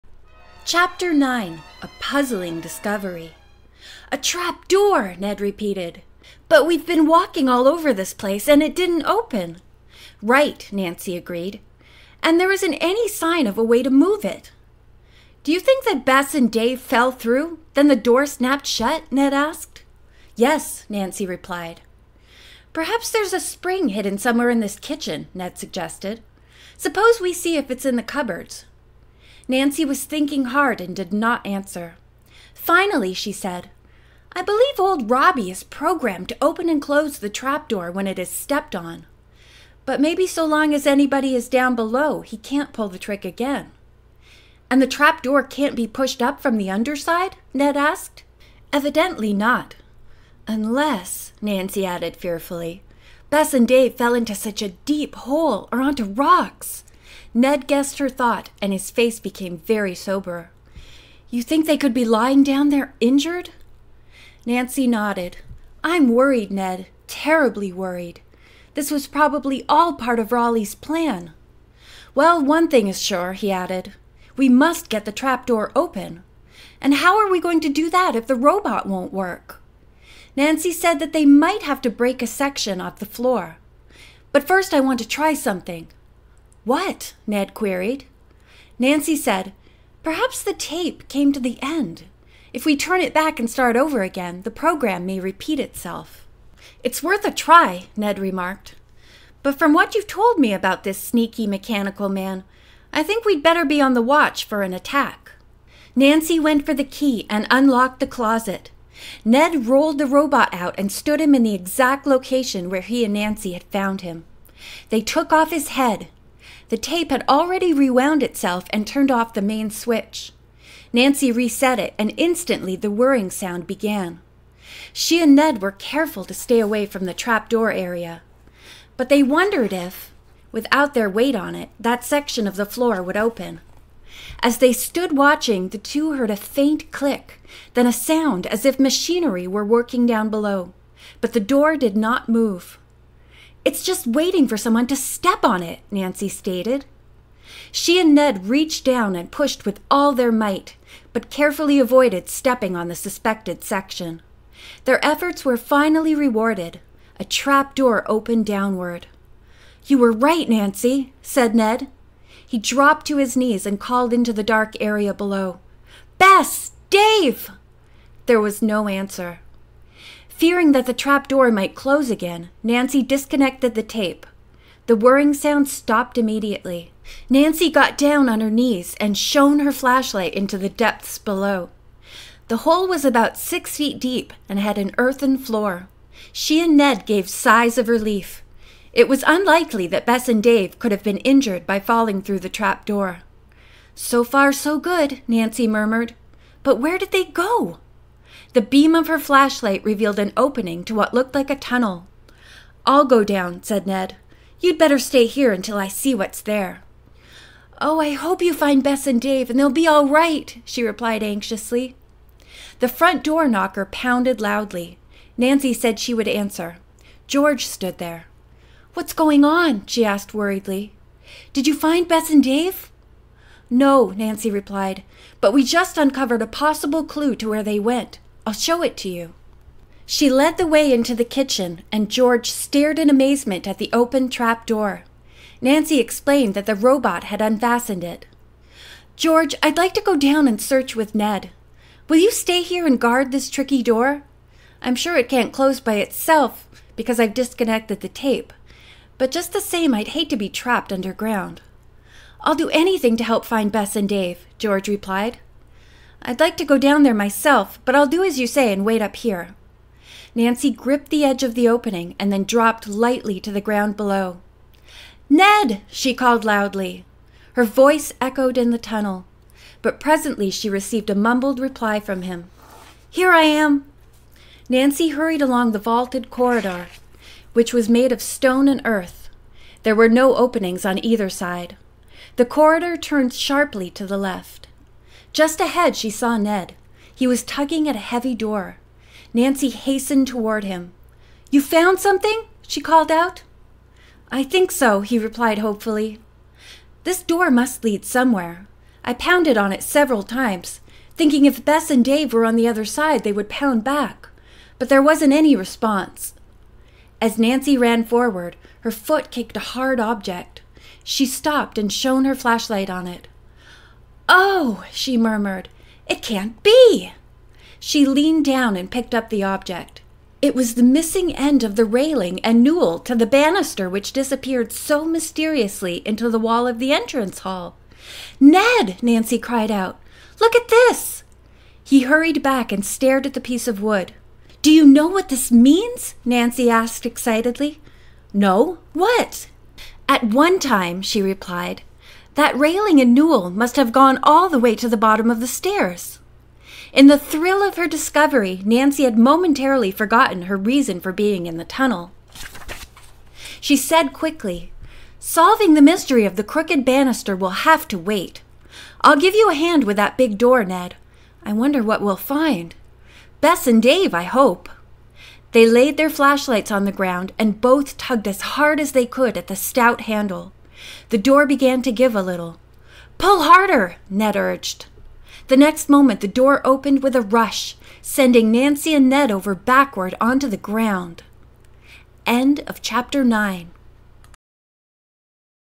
The Crooked Banister // Chapter 9 // A Puzzling Discovery
Once I hit 1000 subscribers, I will be able to do livestreams.

Nancy Drew Mystery Stories #48
The Crooked Banister:

Nancy and her friends Bess and George spend an exciting week exploring a mysterious zigzag house with it's fantastically crooked staircase, it's bizarre serpent picture, and an unpredictable robot that nearly causes the young detective to lose her life. But despite the threat of danger from the robot, Nancy is determined to solve the mystery of the weird house and to locate the missing owner, who is wanted by the police. It takes keen logic and quick-thinking for the young detective to plow through the tangled thicket of clues and find the key to this complex puzzle. With the help of her friends, Nancy captures an elusive swindler and uncovers the secret of the Crooked Banister, but not before they have several hair-raising adventures - one on a broken bridge over flaming water, another in a hidden room with poisoned portraits!

Copyright©️ 1971 Grosset & Dunlap Inc.